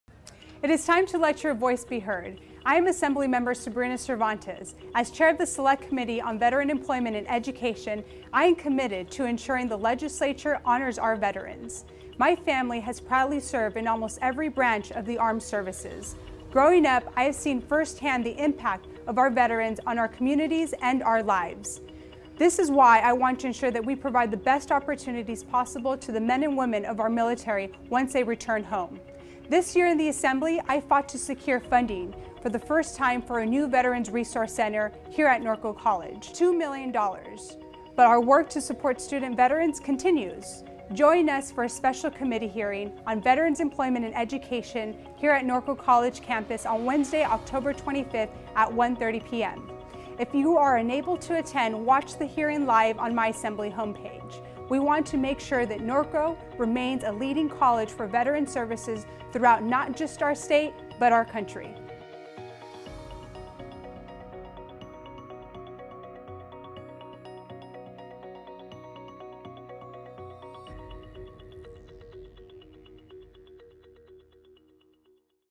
Cervantes Brings Committee Hearing on Student Veterans Issues to Norco College
(Norco, CA) - Join Assemblymember Sabrina Cervantes (D-Corona), Chair of the Select Committee on Veteran Employment and Education, for a special hearing at the Norco College campus at 1:30pm on Wednesday, Oct. 25. Share your experiences and provide lawmakers with your recommendations for ensuring our student veterans receive the best opportunities possible when they return home. If you are unable to attend the hearing, you can follow along via a livestream on my homepage. Find out more in this Assembly Access video.